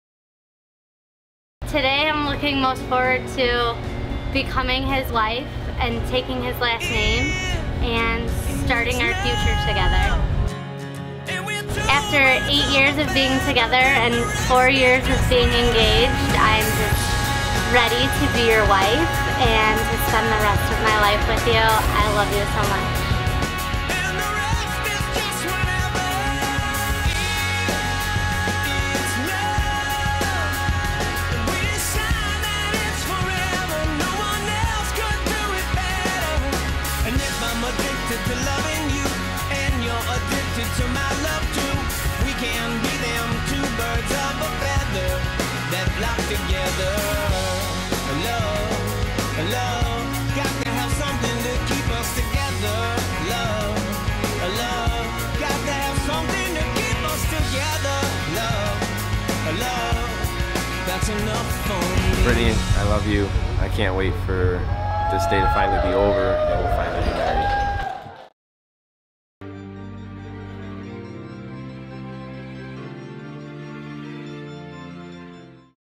September 13 2013 Wedding Film Trailer
This is the trailer to the 1 hr 17 min Wedding Film from Nick and Brittany's Big Day in LaSalle, IL.